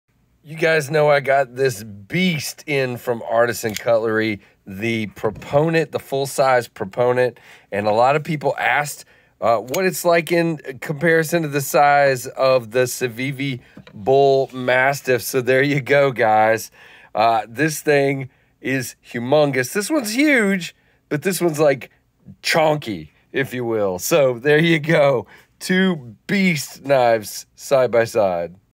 Comparisons of gargantuan proportions!!!!
If you like big knives, I have you covered. between the artisan proponent and the civivi bullmastiff these are awesome. And beasts.

Check out the proponent at my link below.l.